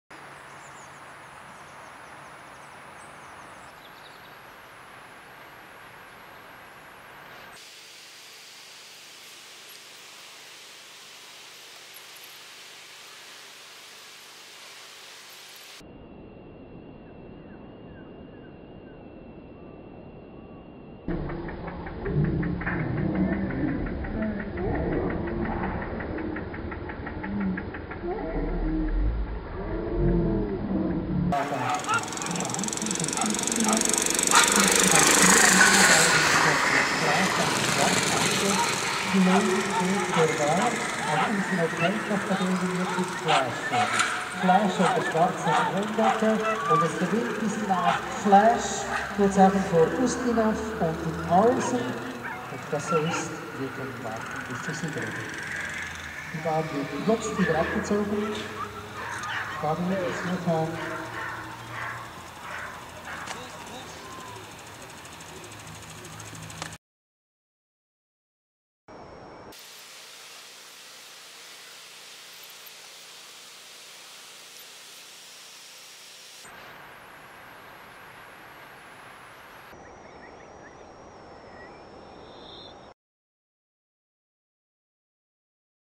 KSM Kleindöttingen 26.10.14: Finallauf Whippet Rüden B-Final (FL 62)
KSM Kleindöttingen, Schweiz